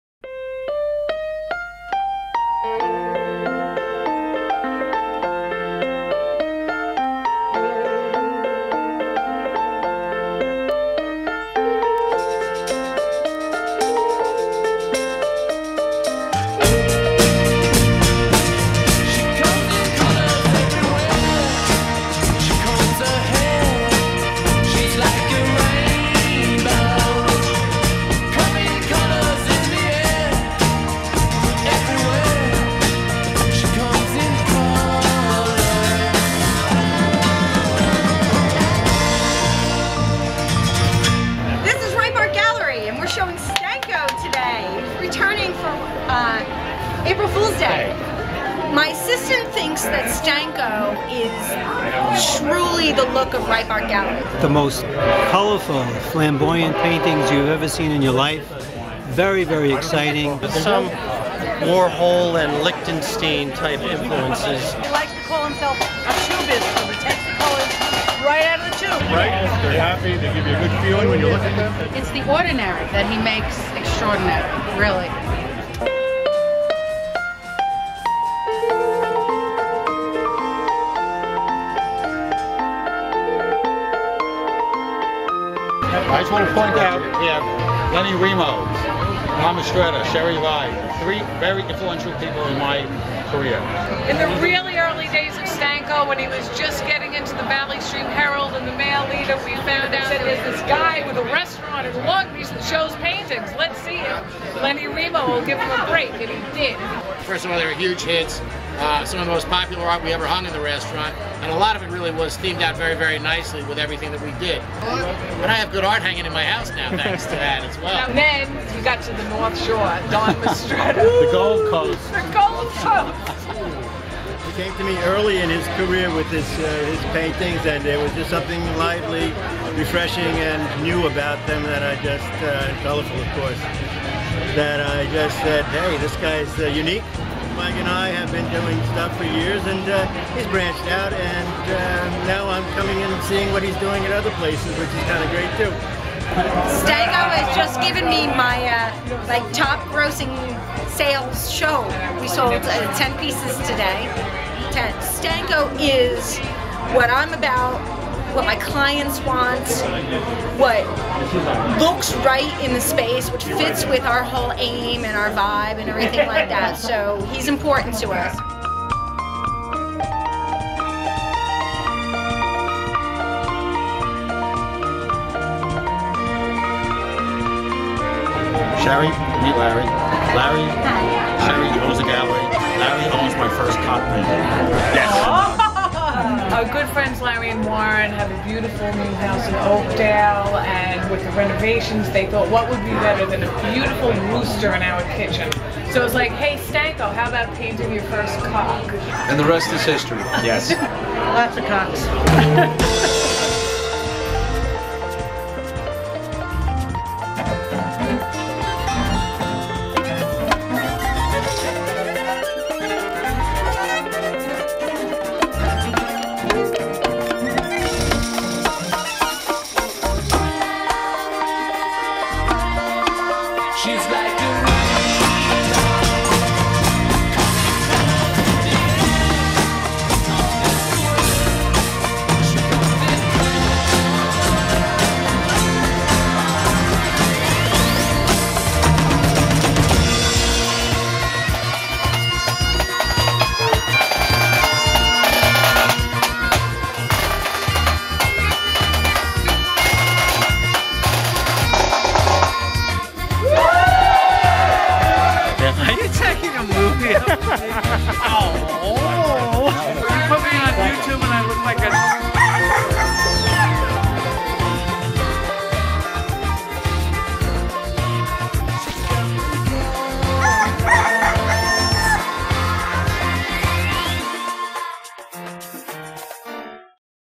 Stanko at Ripe Art Gallery
A short film documenting Stanko's gallery opening at Ripe Art Gallery in Greenlawn, NY.

This video does not intend to infringe on copyright laws by using the selected music in any way.